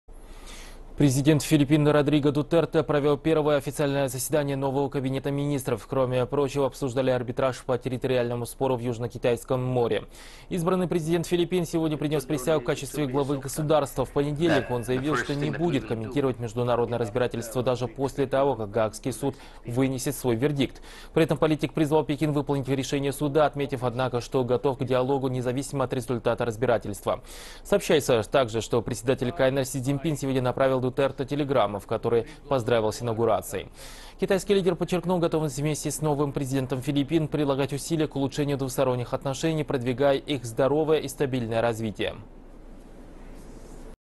Арбитраж по территориальному спору обсудили на заседании нового кабмина Филиппин
Проблема Южно-Китайского моря（Арбитраж）

Телеканал CCTV-русскийЦентральноготелевиденияКитая.
Впрограммепередаютсявсеважныемировыеизвестия, атакженовости, представляющиеинтересдлярусскоязычнойаудитории.